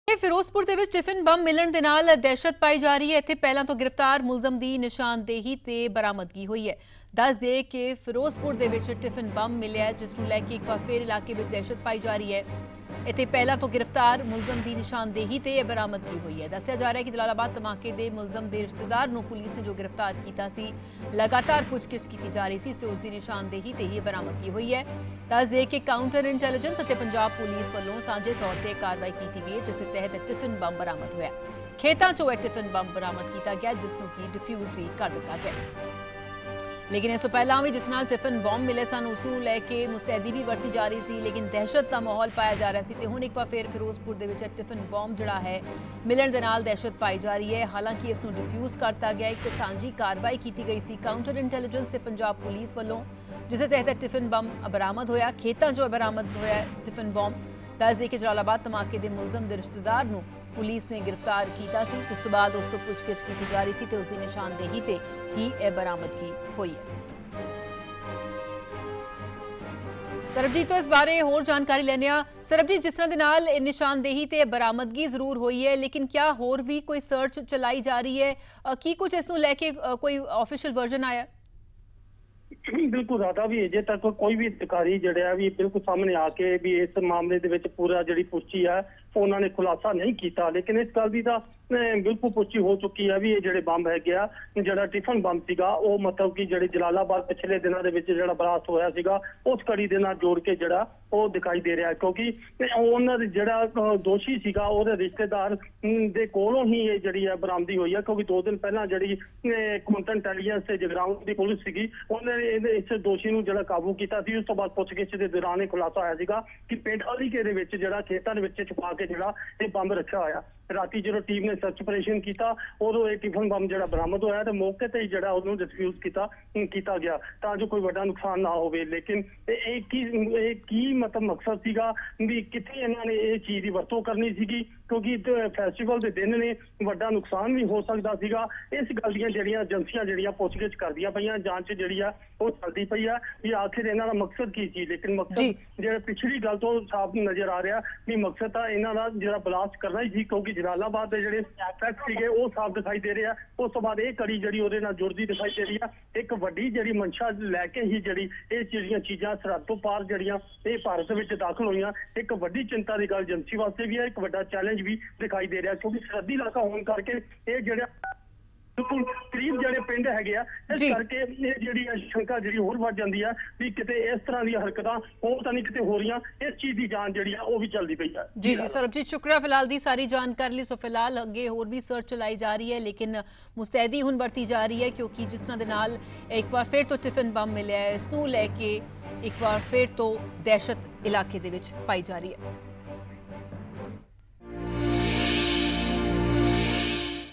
ਫਿਰੋਜ਼ਪੁਰ: ਟਿਫਿਨ ਬੰਬ ਮਿਲਣ ਨਾਲ ਫੈਲੀ ਦਹਿਸ਼ਤ
ਪਹਿਲਾਂ ਤੋਂ ਗ੍ਰਿਫਤਾਰ ਮੁਲਜ਼ਮ ਦੀ ਨਿਸ਼ਾਨਦੇਹੀ ਤੇ ਹੋਈ ਬਰਾਮਦਗੀ।